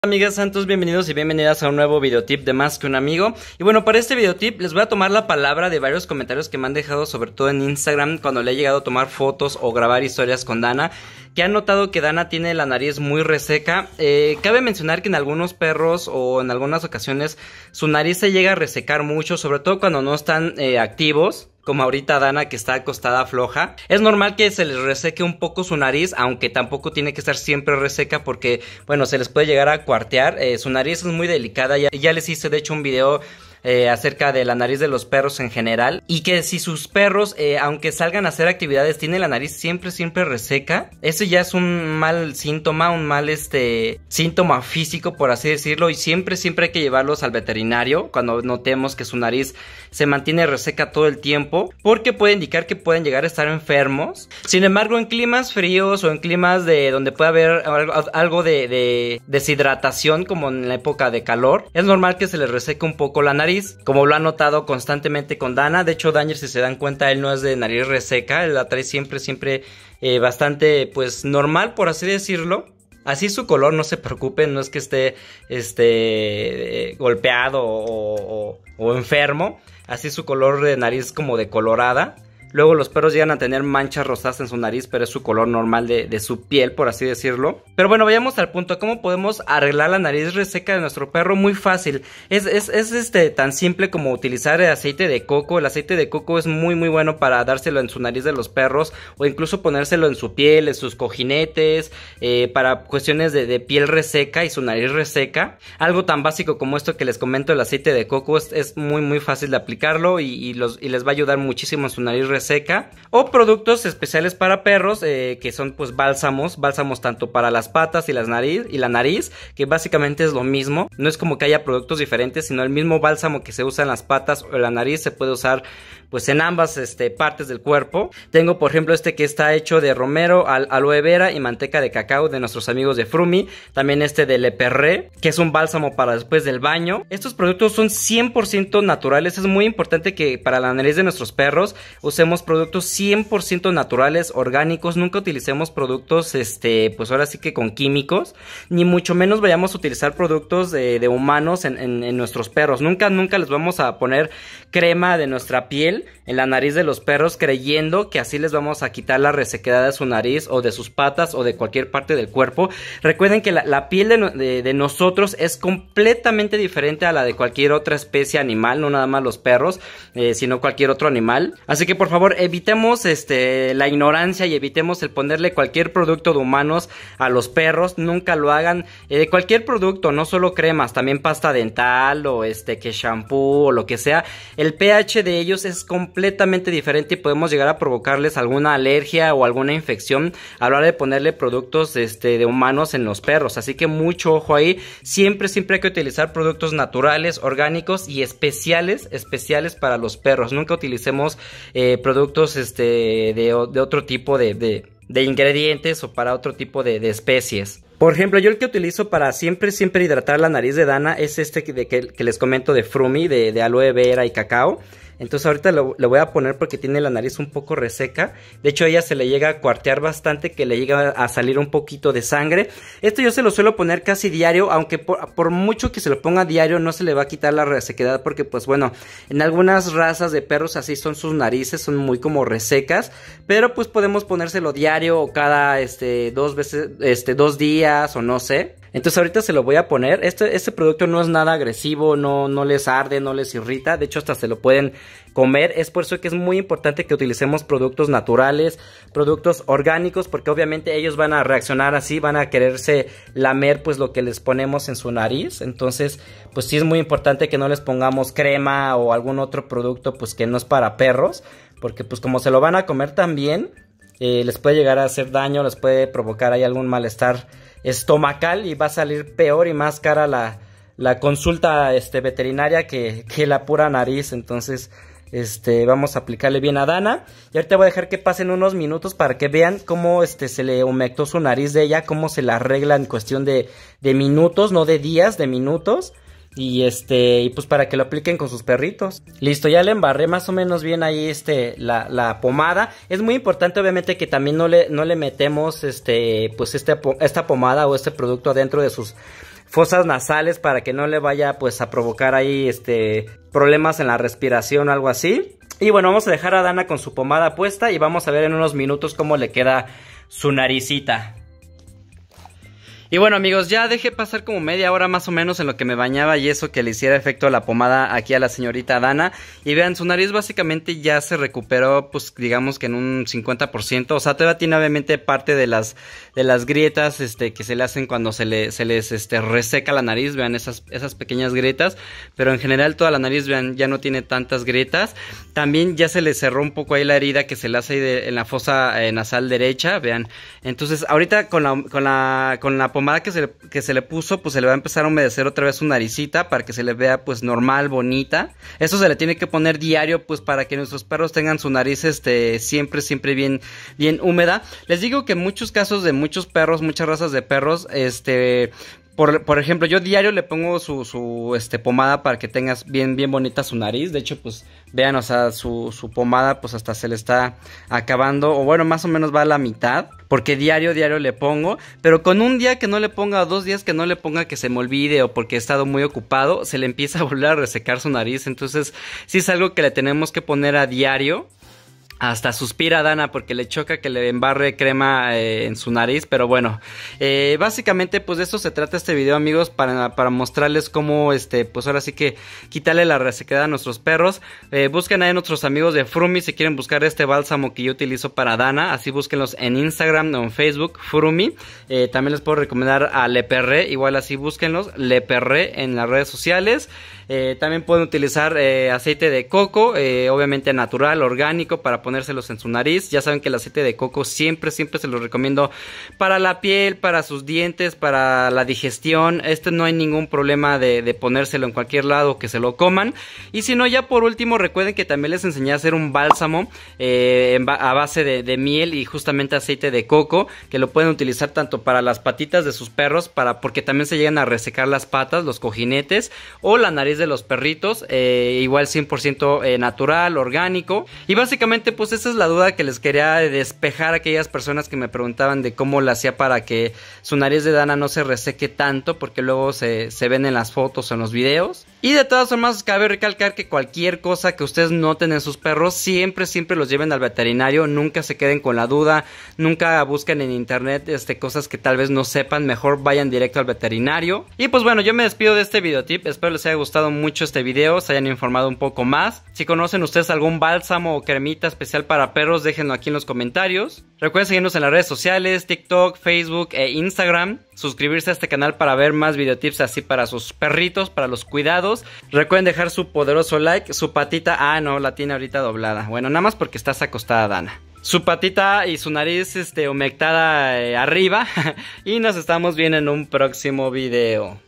Nariz Perfecta: Tips para Curar la Nariz Agrietada de Tu Perro
Descubre cómo curar la nariz agrietada de tu perro con estos consejos útiles. Aprende cómo cuidar las almohadillas agrietadas y mantener la nariz de tu perro saludable. ¡Tu mascota te lo agradecerá!

00:57 Nariz reseca es mal sintoma
1:42 Porque se decolora la nariz
1:49 Como curar la nariz
2:40 No usar productos de humanos